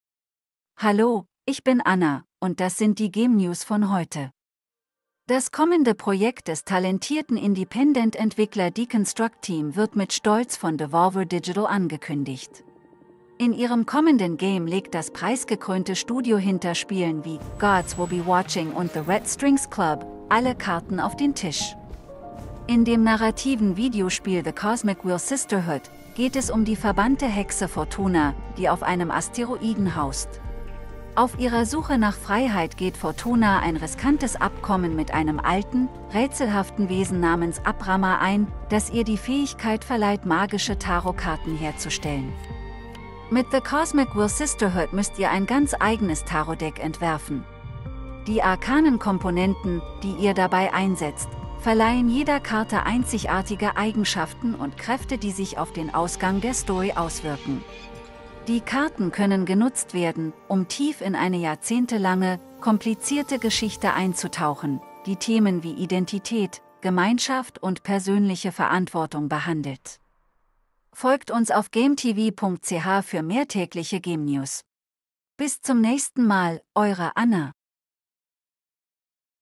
Daily GameNews: COSMIC WHEEL SISTERHOOD
Das kommende Projekt des talentierten Independent-Entwickler Deconstructeam wird mit Stolz von Devolver Digital angekündigt. In ihrem kommenden Game legt das preisgekrönte Studio hinter Spielen wie "Gods Will Be Watching" und "The Red Strings Club" alle Karten auf den Tisch.
In dem narrativen Videospiel "The Cosmic Wheel Sisterhood" geht es um die verbannte Hexe Fortuna, die auf einem Asteroiden haust. Auf ihrer Suche nach Freiheit geht Fortuna ein riskantes Abkommen mit einem alten, rätselhaften Wesen namens Abramar ein, das ihr die Fähigkeit verleiht, magische Tarotkarten herzustellen.

Mit The Cosmic Wheel Sisterhood müsst ihr ein ganz eigenes Tarotdeck entwerfen. Die arkanen Komponenten, die ihr dabei einsetzt, verleihen jeder Karte einzigartige Eigenschaften und Kräfte die sich auf den Ausgang der Story auswirken. Die Karten können genutzt werden, um tief in eine jahrzehntelange, komplizierte Geschichte einzutauchen, die Themen wie Identität, Gemeinschaft und persönliche Verantwortung behandelt.

►Sendeplatz auf Swisscom TV